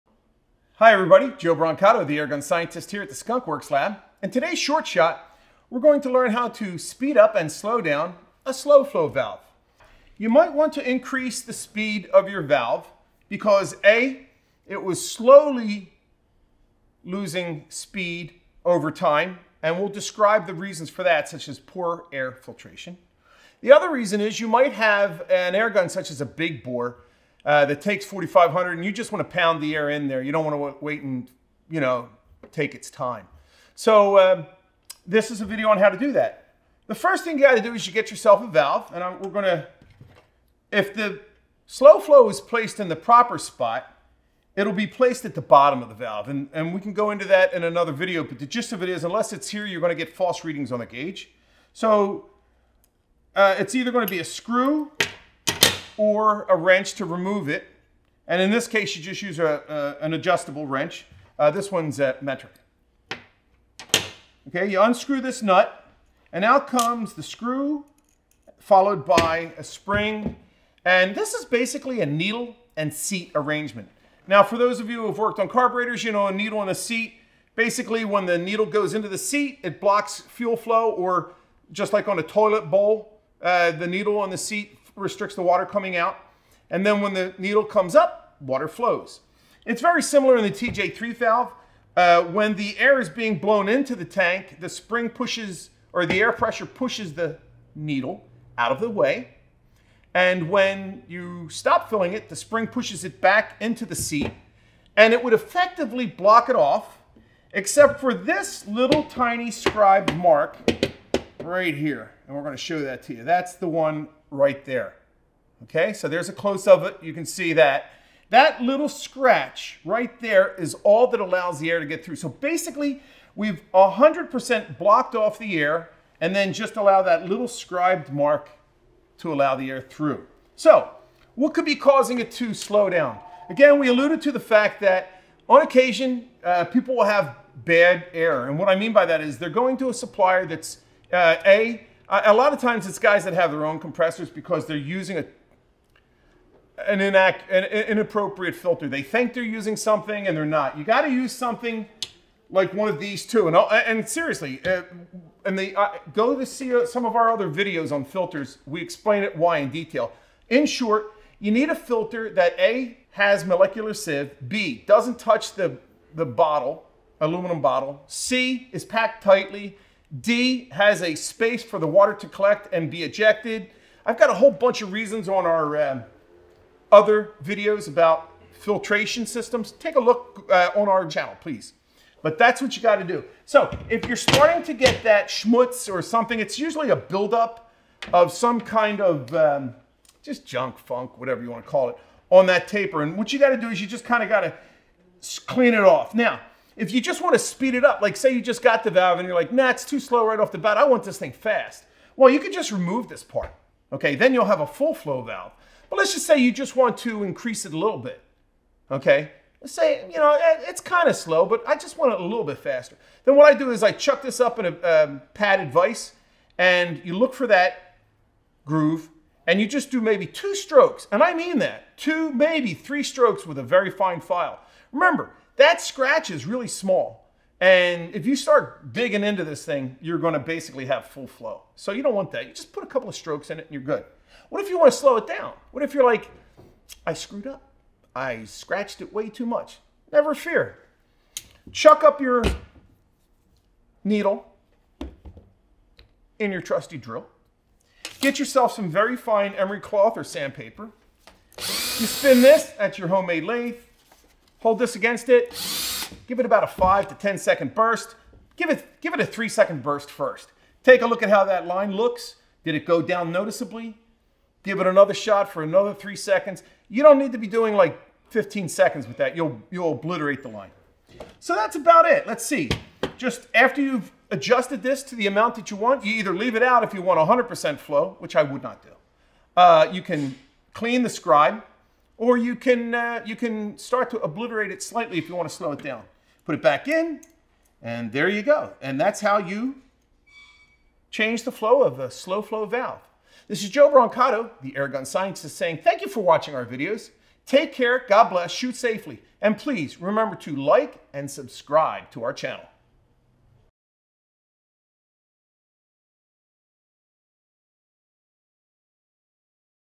Short Shots EP5 : Adjusting and Maintaining Your Slow-Flow Valves
This week we are going to dive into how to adjust and maintain your Slow-Flow valves on your airtank valves.  Its fairly important to keep it in check so you get the right filling speeds, and super beneficial to be able to customize it to your liking.

for any products/questions you may have.